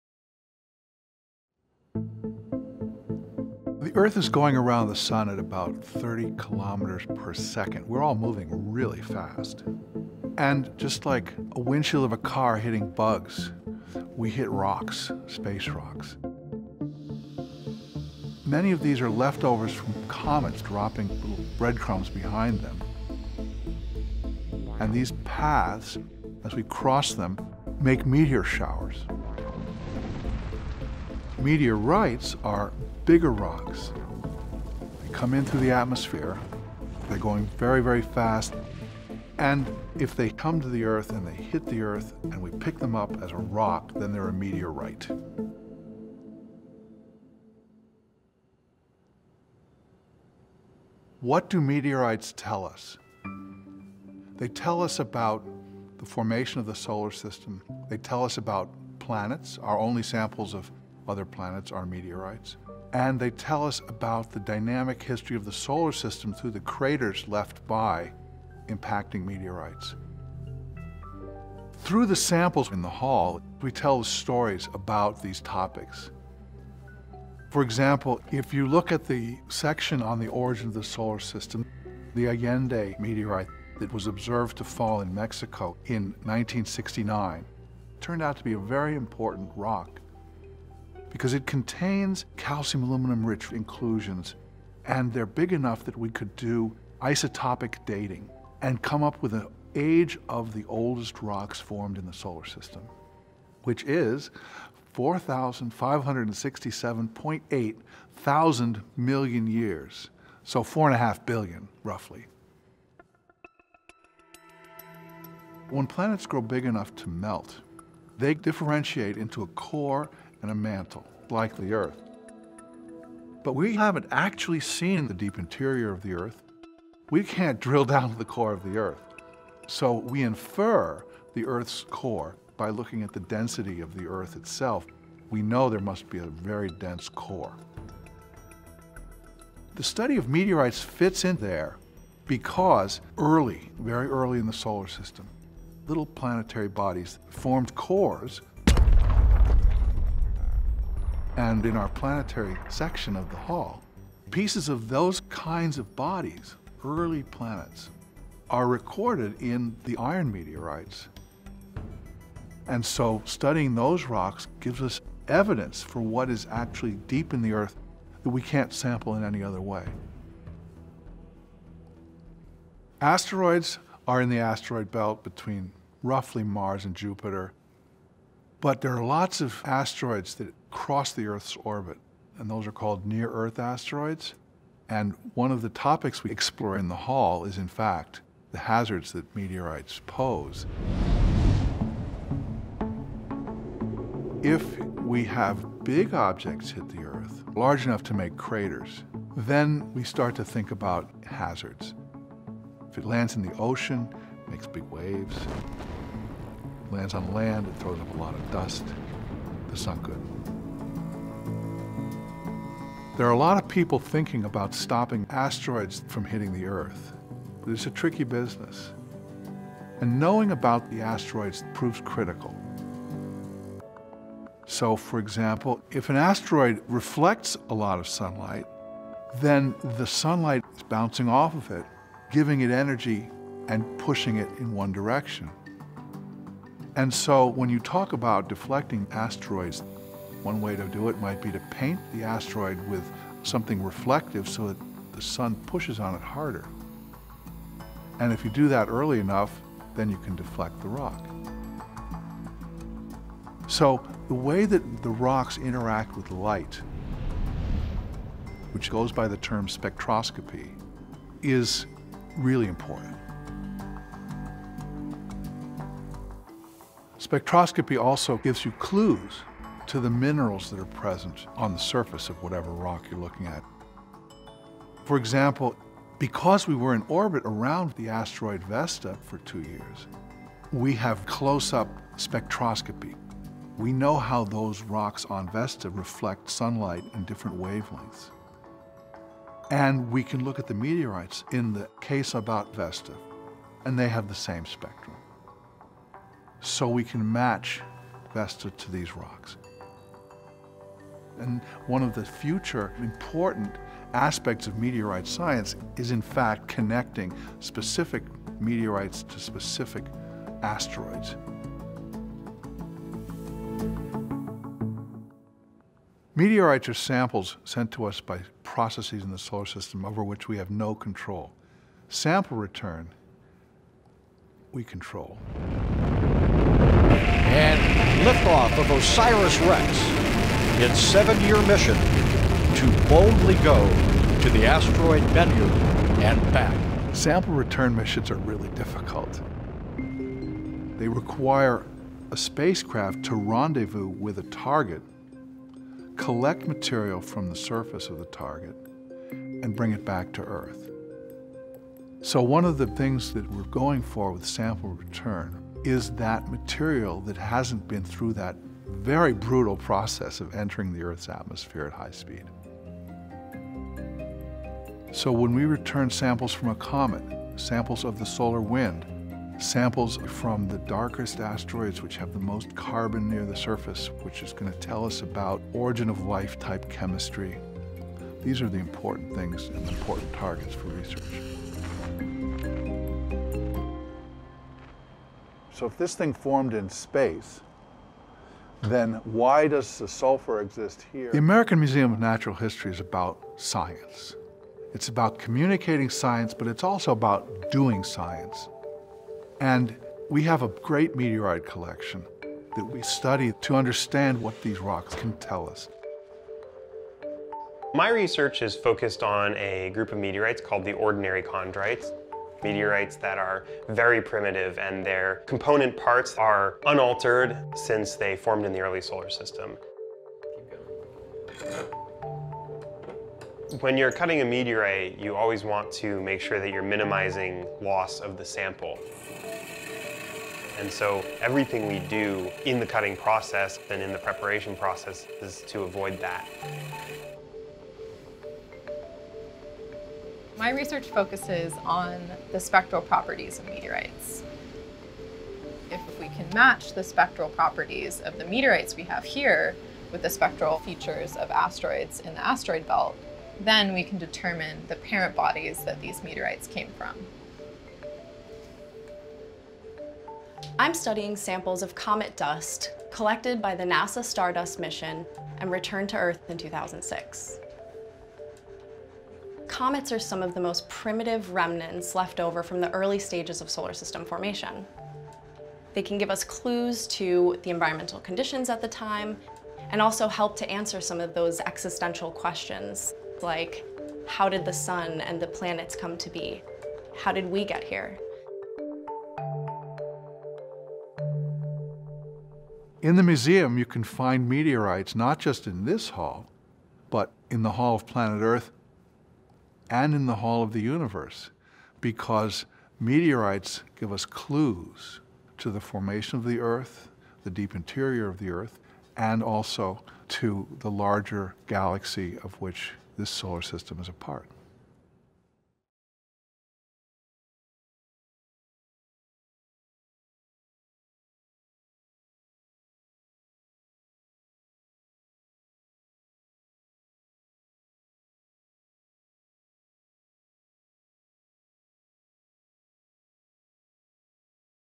☄️☄️Explore the Arthur Ross Hall of Meteorites! ☄️☄️
As the Earth rotates around the Sun, rock fragments enter our atmosphere and fall to the surface where we can collect them as meteorites. What can we learn from these space rocks? They hold data about the formation of the solar system and planets. Curator in the Division of Physical Sciences Denton Ebel takes us on a tour of the Arthur Ross Hall of Meteorites at the American Museum of Natural History where we can explore the origins of the solar system and planets, learn about sample-return missions, and delve into hazards posed by meteorite impacts.

...and for more information on the Arthur Ross Hall of Meteorites